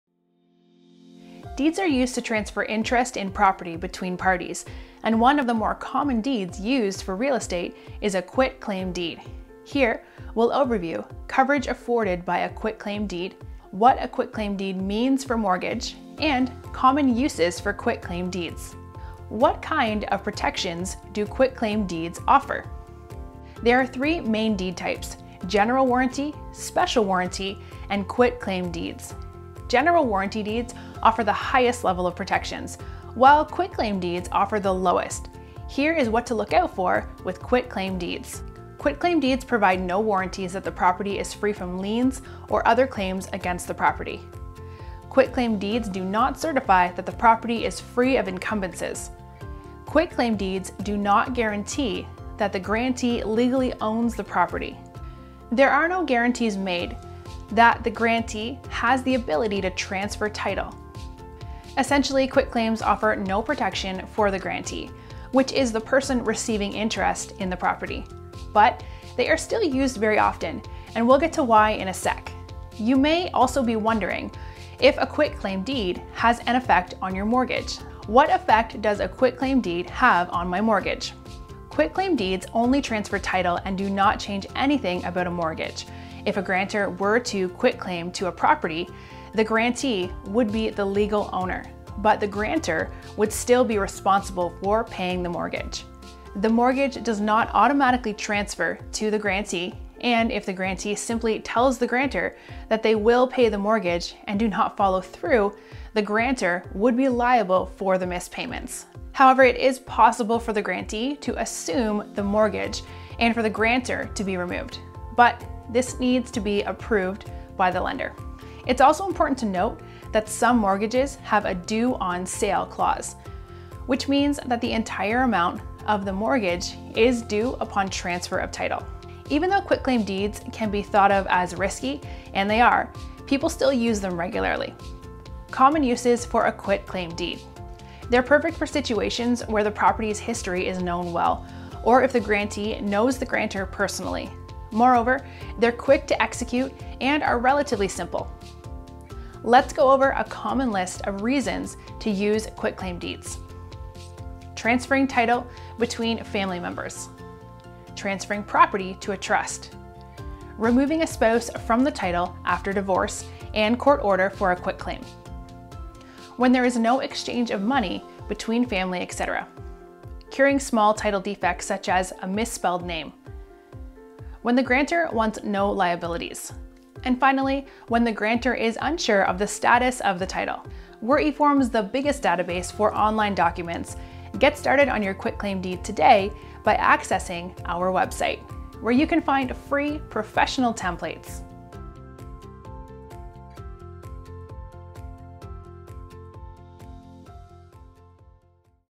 What is a Quitclaim Deed? - EXPLAINED
Quitclaim deeds transfer property from one person to another. They also can remove someone’s name from a deed by having one party “quit claim” to the property.

But while quitclaim deeds are one of the most common deeds used in real estate, they shouldn’t be used in certain transactions. Watch our video to find out more about quitclaim deeds and appropriate use cases.

Read more about quitclaim deeds by accessing the article above (and find a link to make your own deed).

0:00 Intro
0:11 We'll Overview
0:20 What Kind Of Protections Do Quitclaim Deeds Offer
1:19 Effects On Mortgage
2:16 Common Uses For Quitclaim Deed